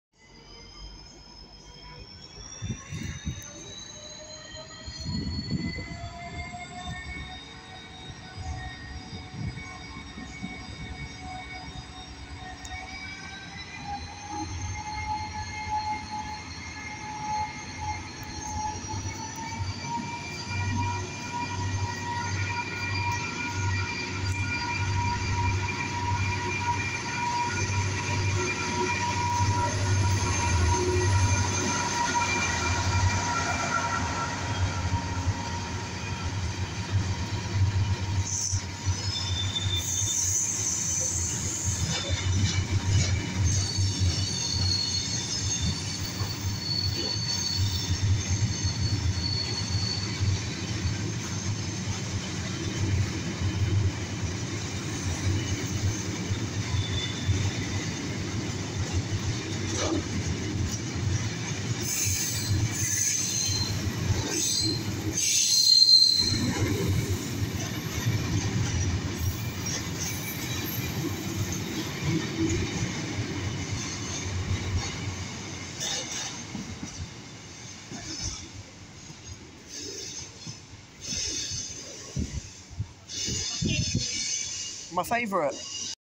Class 59002 | "Alan J Day" (Yeoman Enterprise) | Salisbury | 18/04/2022
I also recorded this in a previous vid on my camera. This is the phone footage, enjoy.